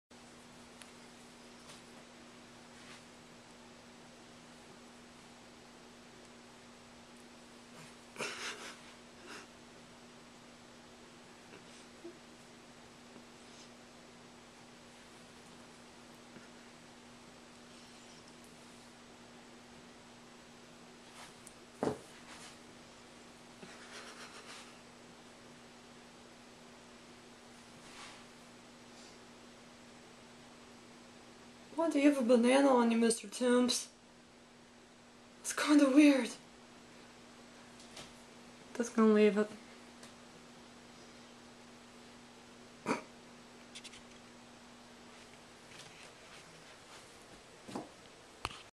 Cat with a Banana on his Head
My cat with a banana on his head. Style and class.